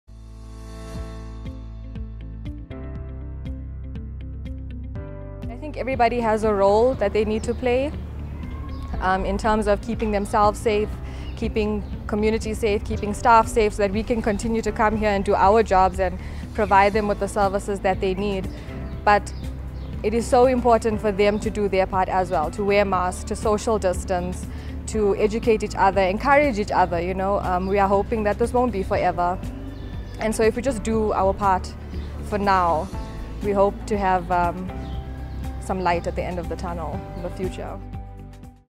ListenToTheDoctorEveryone has a role to play says Dr Dirushka Naidoo, dentist at Alex CHC.
Everyone has a role to play in containing the spread of Covid19, says Dr Dirushka Naidoo, dentist at Alex CHC.